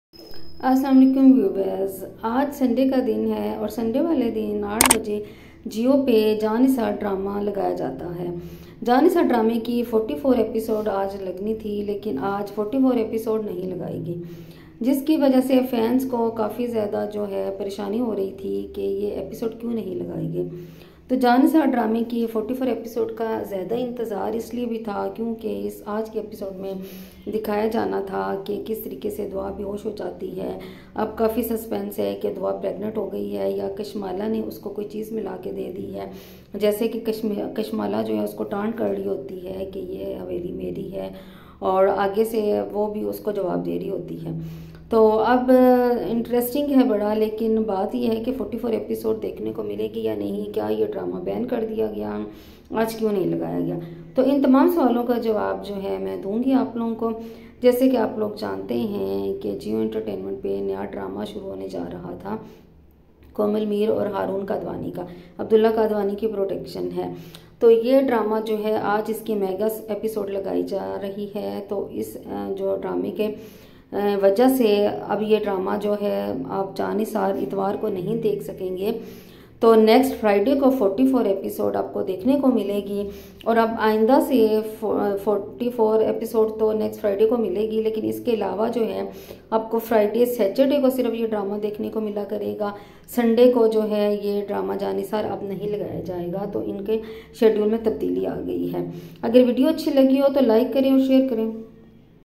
Jaan nisar episode 44 || New update ||Courtesy har pal geo
Jaan nisar episode 44||new update jaan nisar drama

Jaan nisar episode 44 is not uploaded .Fans are curious to know .New update about jaan nisar drama is that from now it will not come on sunday.Instead of it new drama ishq hua will be uploaded .

All images and pics belongs to its owner .